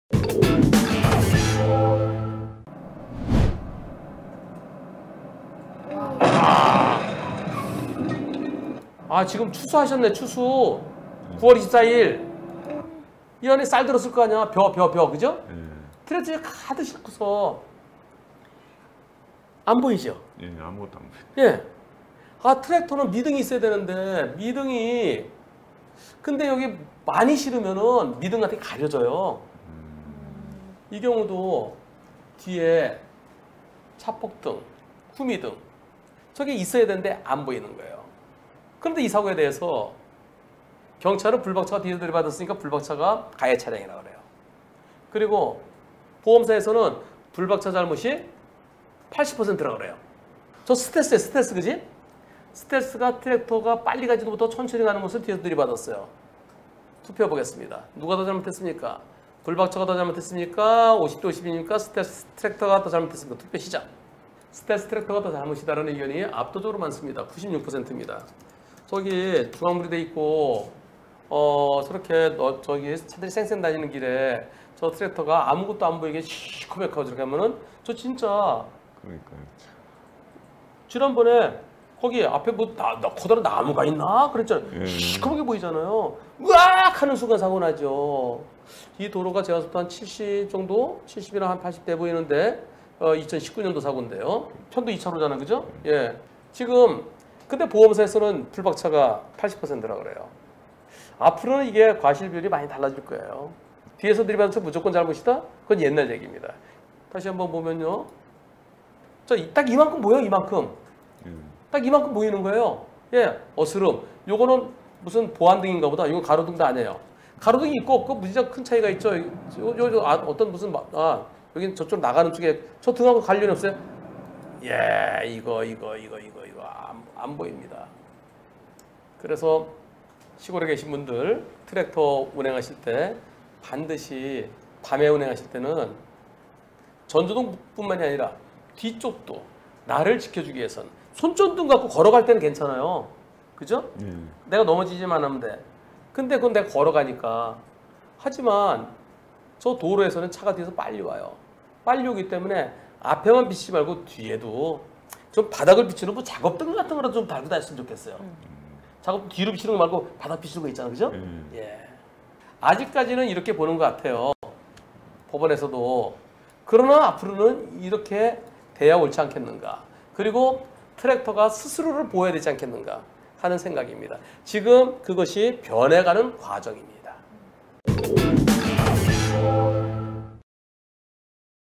12079회. 미등도 잘 보이지 않는 트랙터와 사고 났는데 블박차 과실이 80%?
b3391, u2460, 210723 (금) 오후 생방송, 스텔스 트랙터

밤에 미등이 잘 보이지 않는 트랙터와 추돌사고.

경찰은 블박차 잘못이 더 크다고 함,
블박 보험사에서는 블박차 과실 80% 주장.

* 투표
1. 블박차가 더 잘못 (4%)
2. 50:50
3. 스텔스 트랙터가 더 잘못 (96%)

* 트랙터 밤에 운전할 땐 전도등 뿐 아니라 뒤쪽도.